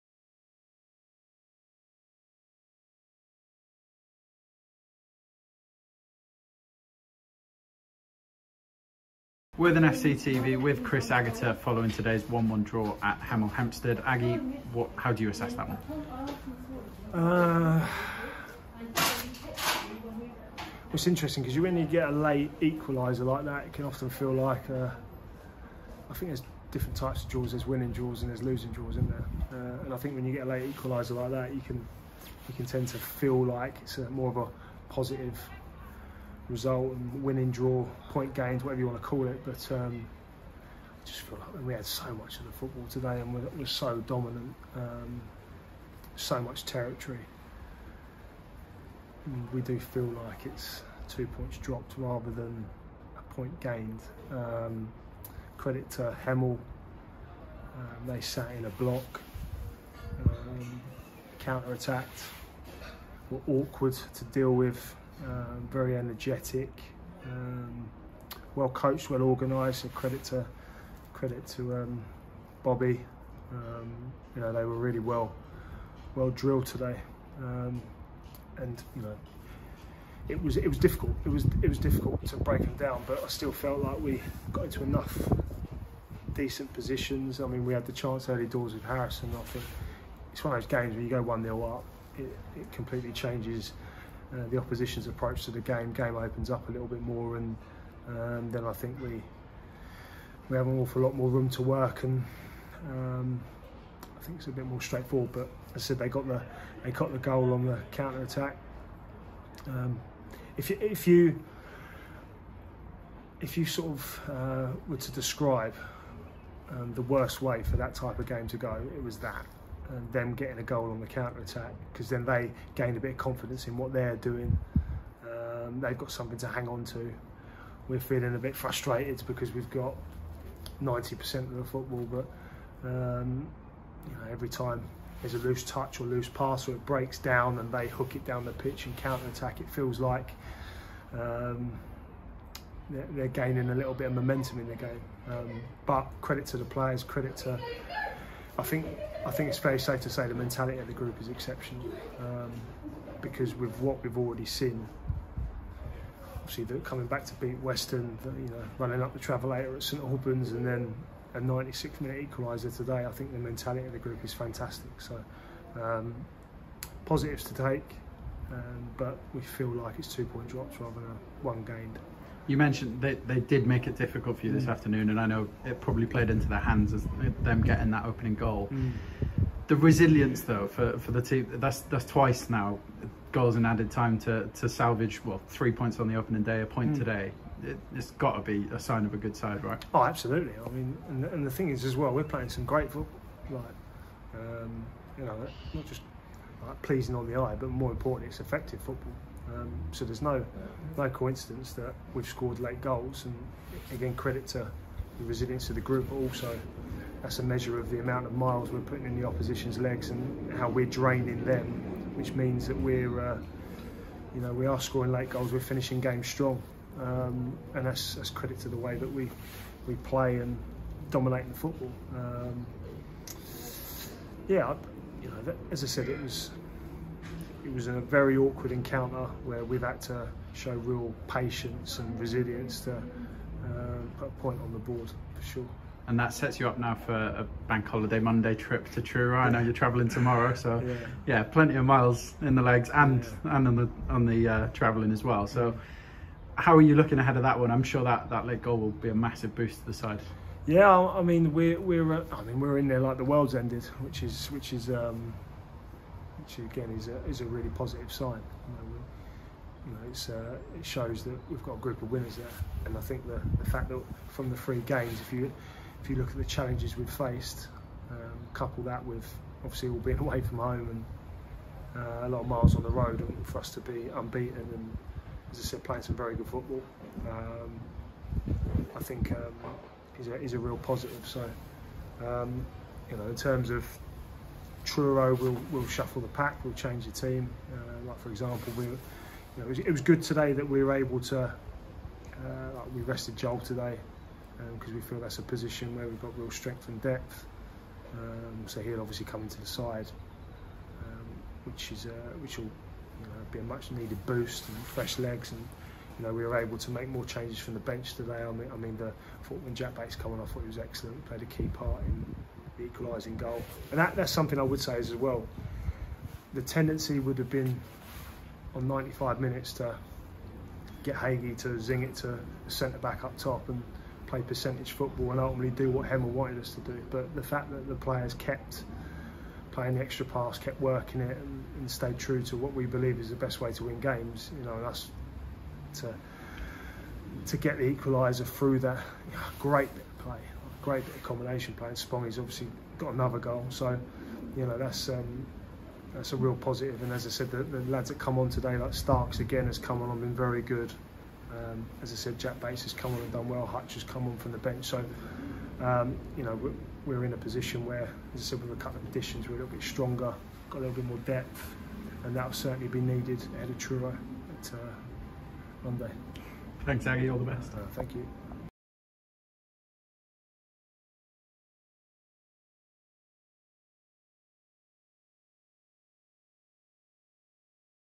"We do feel like it's two points dropped" | Chris Agutter | Hemel Hempstead Town Reaction
The boss reflects on today's point on the road at Hemel Hempstead.